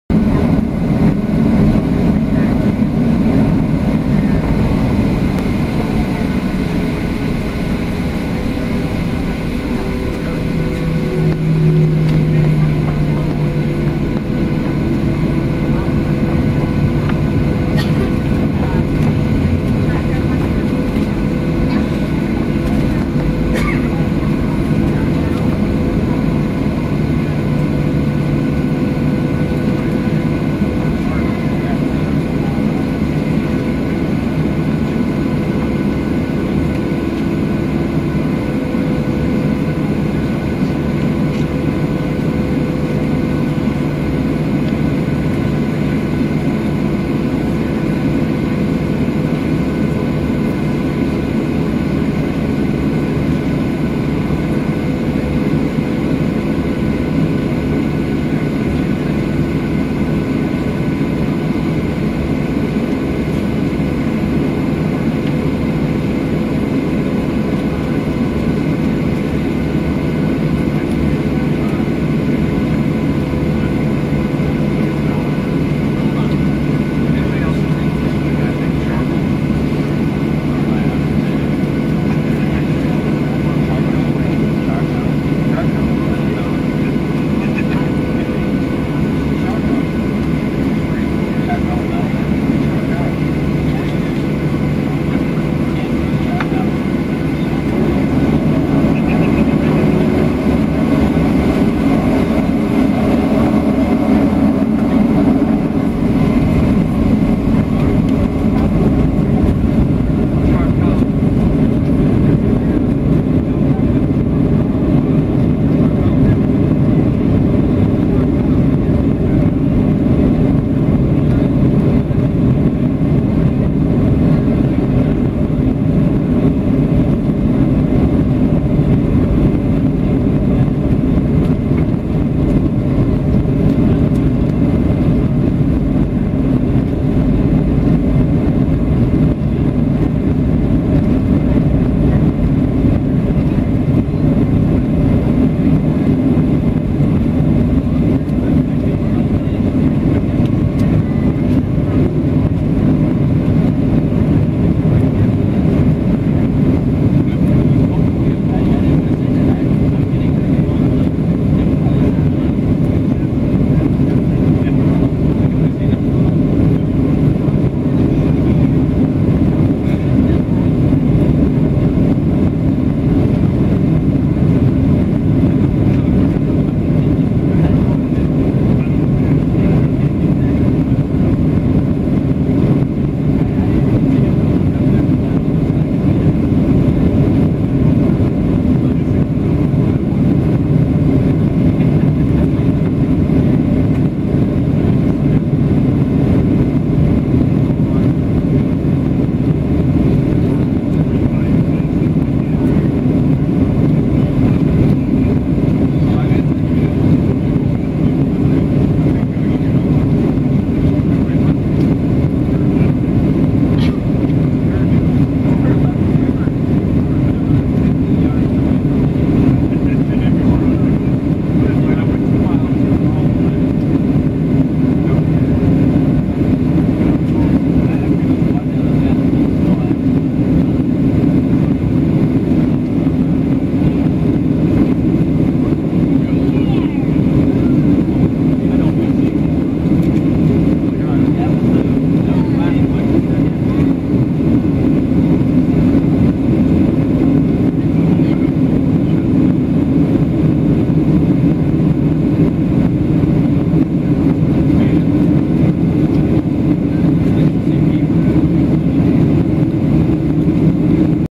Southwest Airlines Boeing 737-700 Takeoff from Baltimore-Washington International Airport
On a flight from BWI-BDL. We took off on runway 28. Click here for more info on the aircraft.

Aircraft:
Type: Boeing 737-700
Tail Number: N229WN
Delivery Date: June 30, 2006